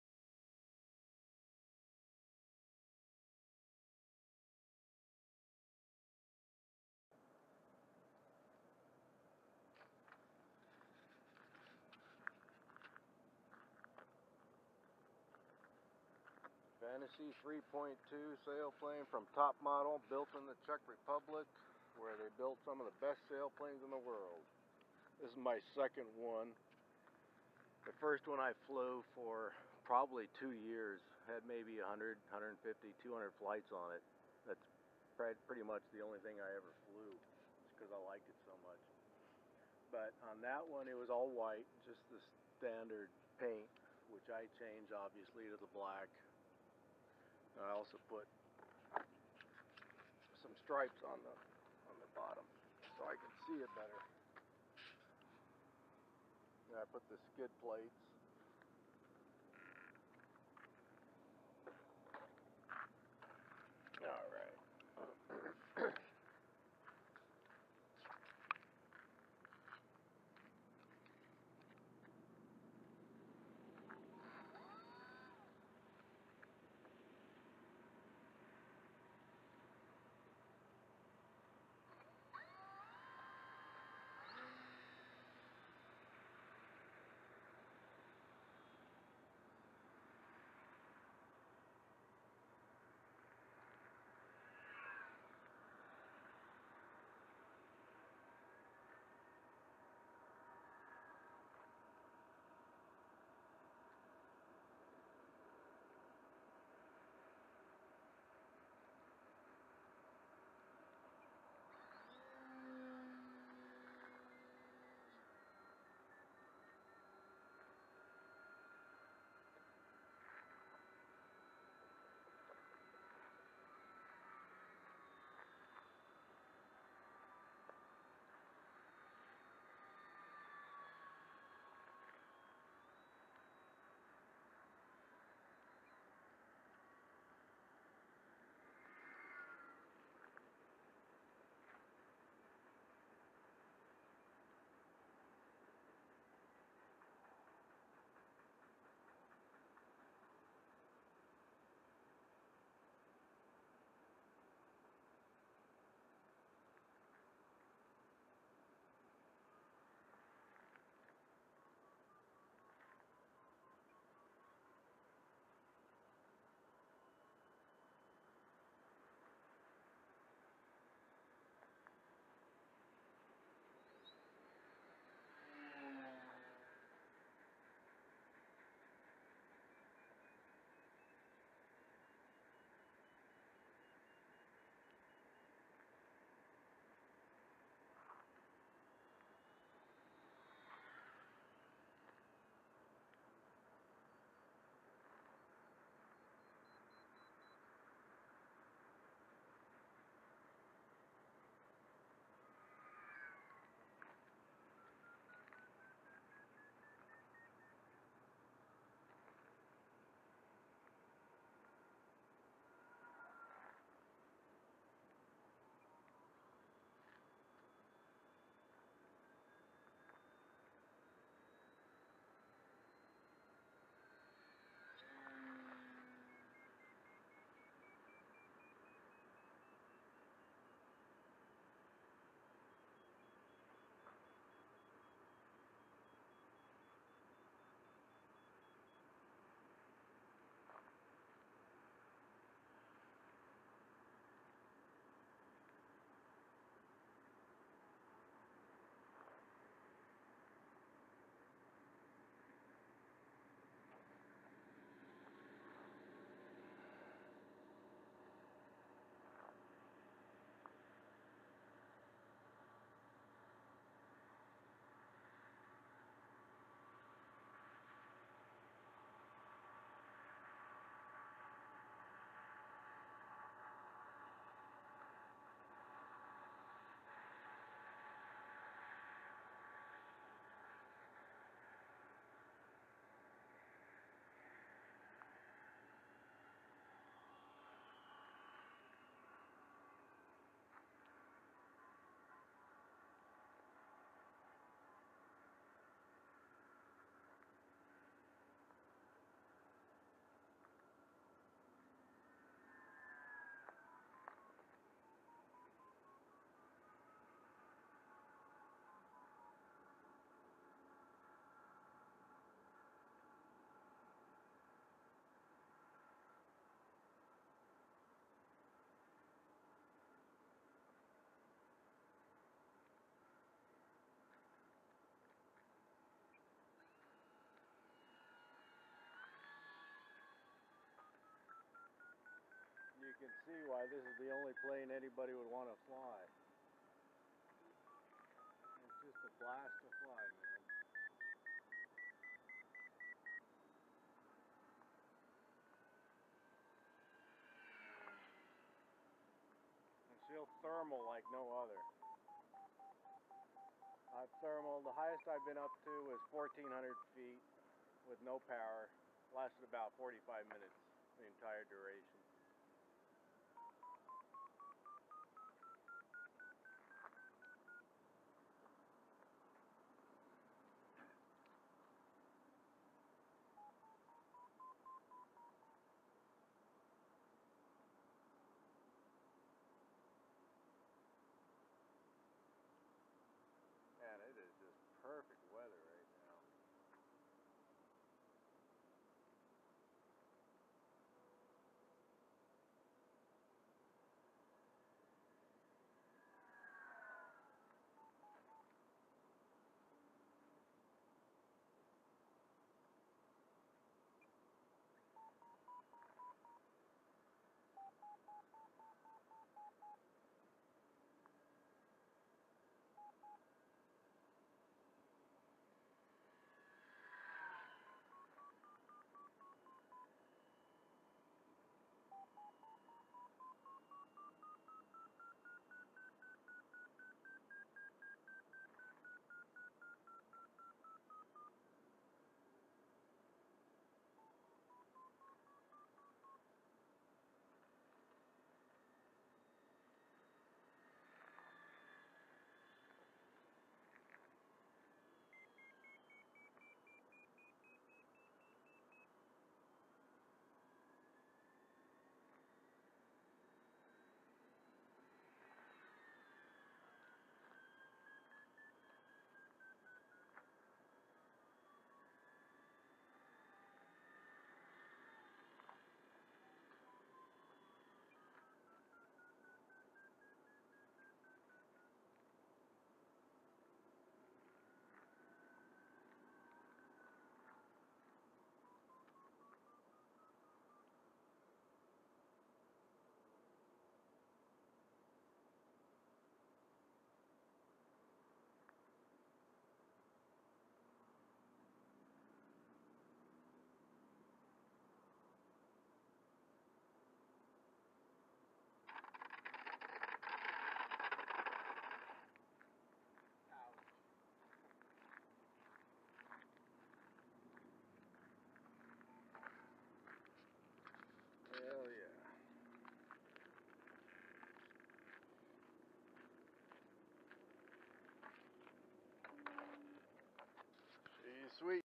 Fantasy 3 2
Sport flying the Fantasy 3.2 meter sail plane. Runcam audio sucks man :(